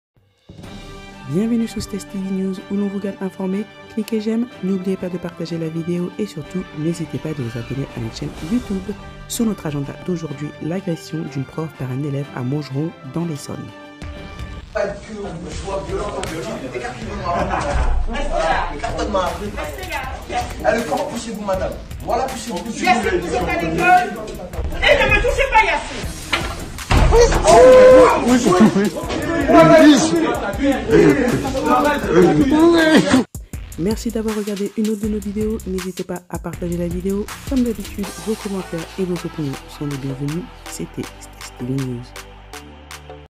[STESS TV News] L' Agression D'une Prof à Combs-la-ville Par Un Élève
[STESS TV News] Essonne : L' Agression D'une Prof à Combs la ville Par Un Élève